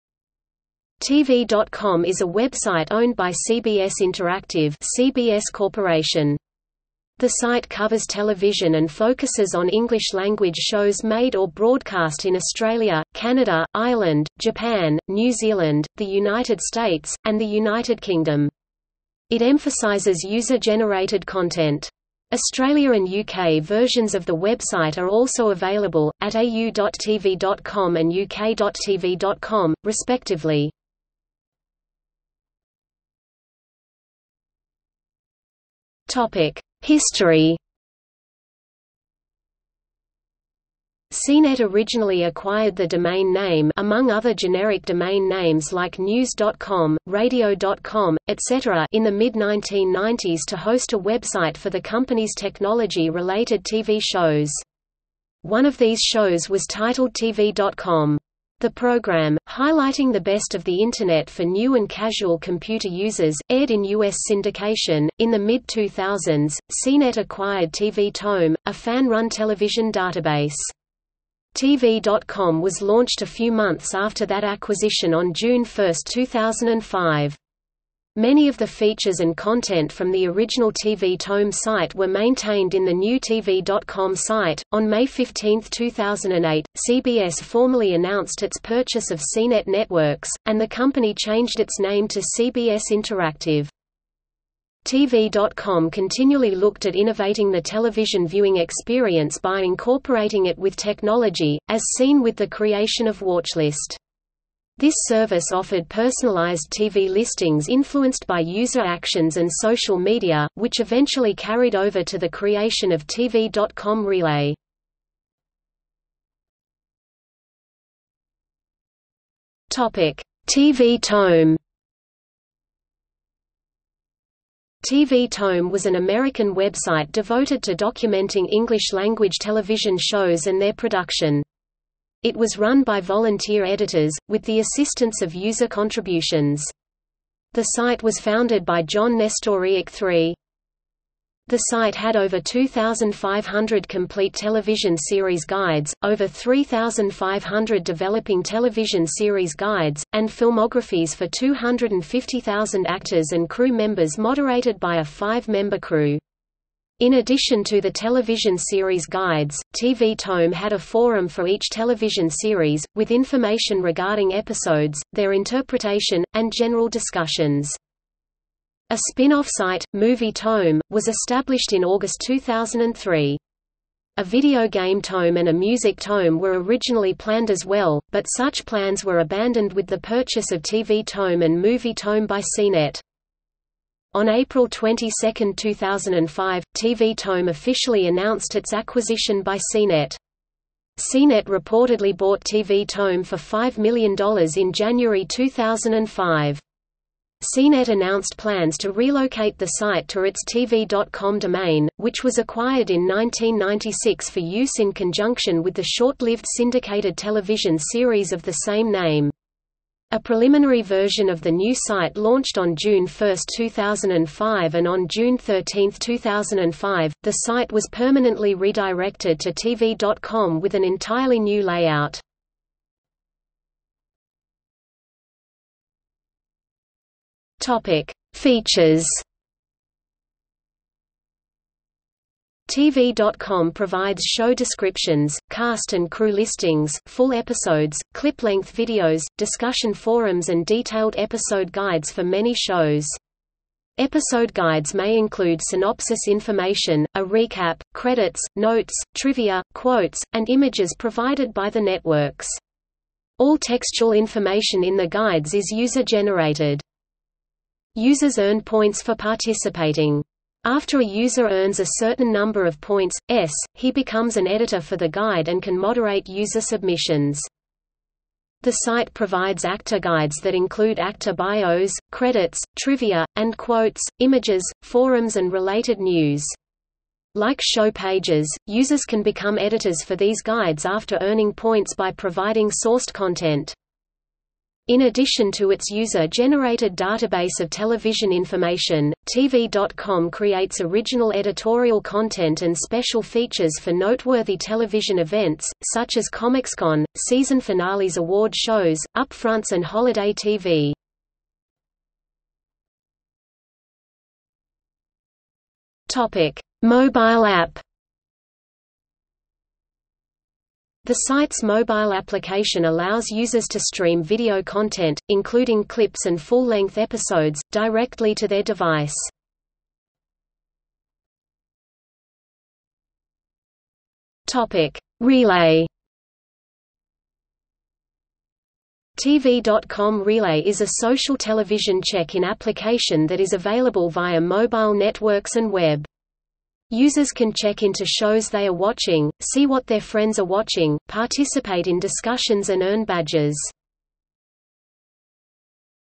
TV.com | Wikipedia audio article
00:00:32 1 History
00:01:56 1.1 TV Tome
00:03:44 2 Features
00:05:02 2.1 Mobile app
00:05:21 2.2 Relay

- Socrates

SUMMARY
TV.com is a website owned by CBS Interactive (CBS Corporation). The site covers television and focuses on English-language shows made or broadcast in Australia, Canada, Ireland, Japan, New Zealand, the United States, and the United Kingdom. It emphasizes user-generated content. Australia and UK versions of the website are also available, at au.tv.com and uk.tv.com, respectively.